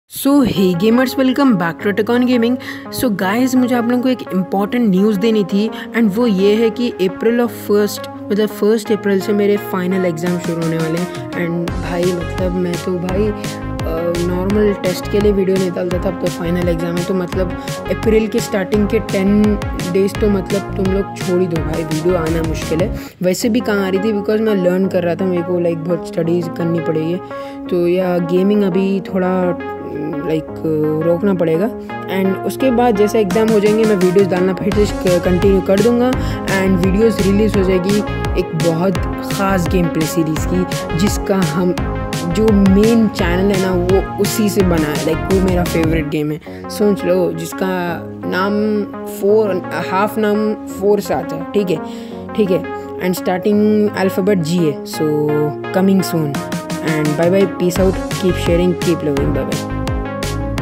Exams?why?videos ? gsv4? must watch..!
hi guys i want to tell that i cant upload vids because of my exams so pls dont think i left youtube i will back at 10th april so pls keep supporting me love you all..!

have a good time with gaming..!